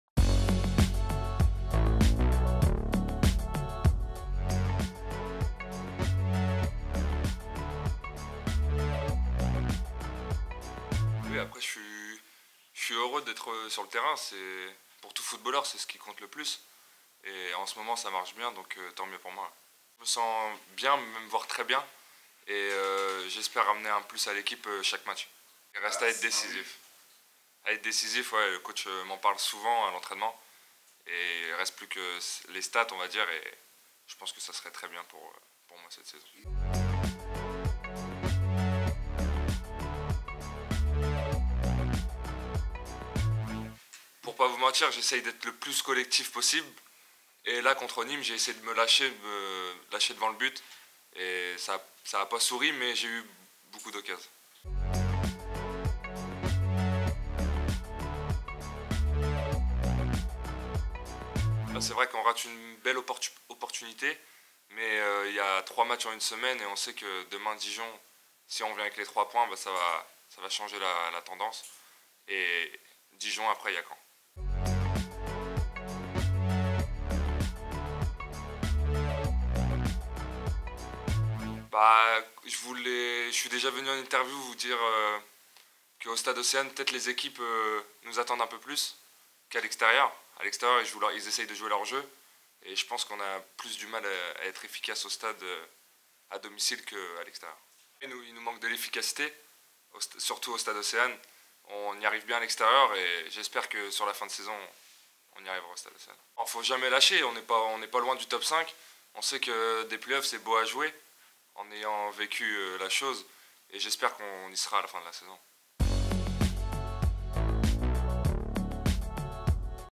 💬 Dijon - HAC : La conférence de presse d'avant match avec Himad Abdelli
Avant Dijon - HAC, interview du milieu de terrain havrais Himad Abdelli…